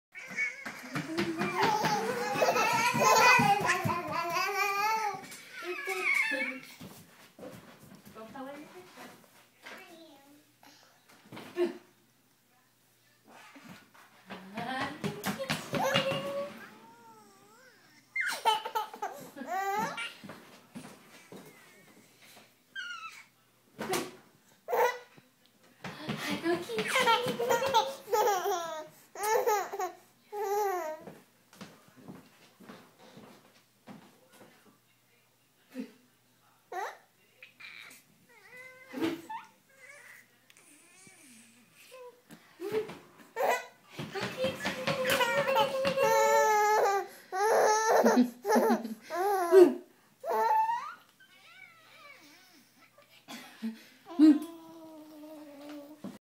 Alaina giggles
During dinner time Alaina thought mommy was very funny!